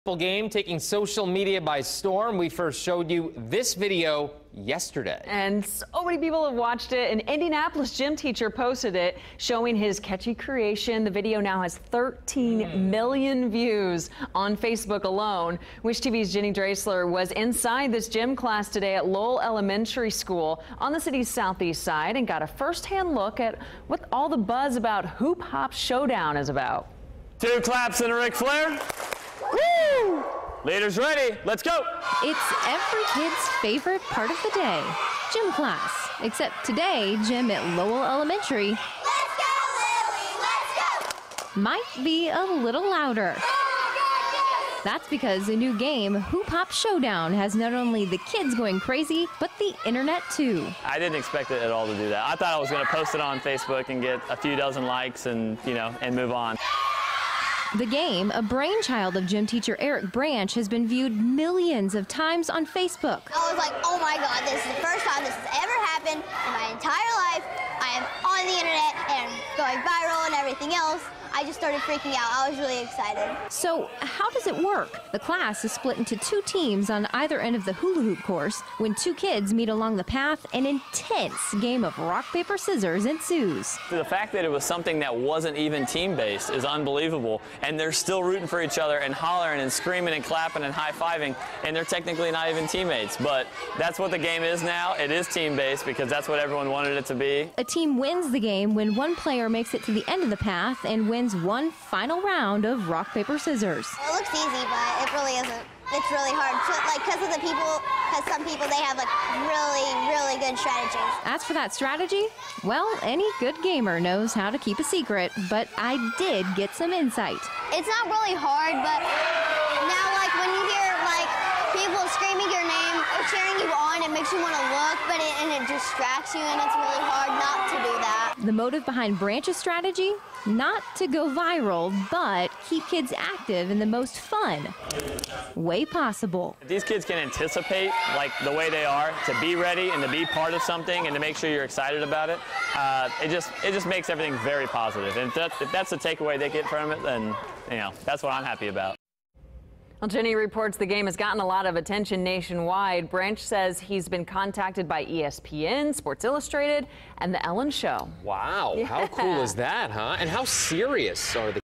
Indy gym teacher shocked by success of viral 'Hoop Hop Showdown' video
News 8's Jenny Dreasler reports.